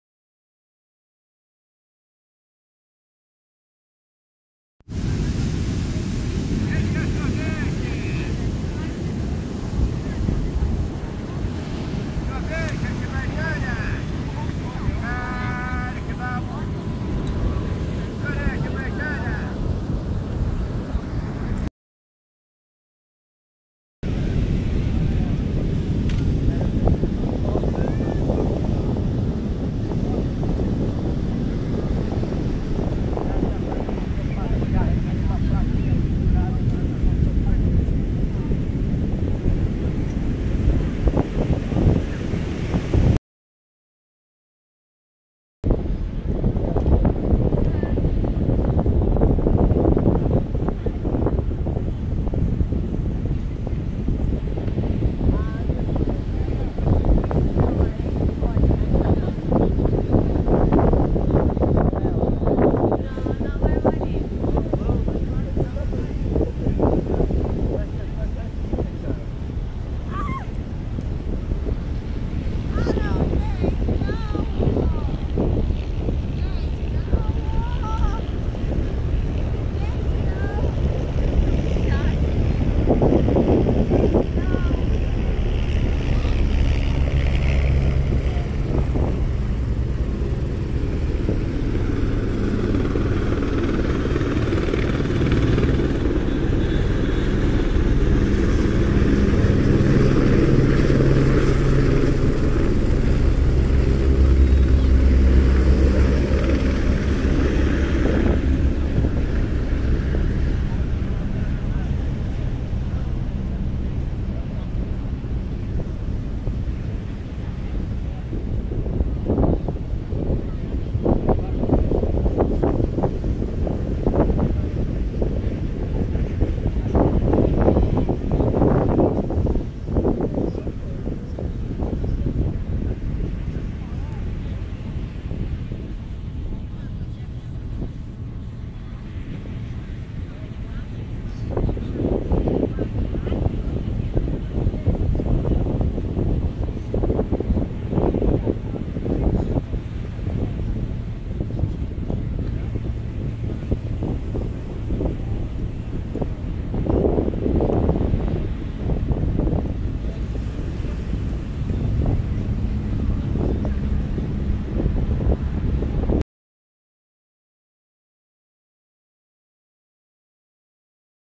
Novo modo de usar Drones Marketing voador nas areias da Praia de Ipanema
Campanha do restaurante Subway com uma placa pendurada no Drone oferecendo Wi-fi Gratis!!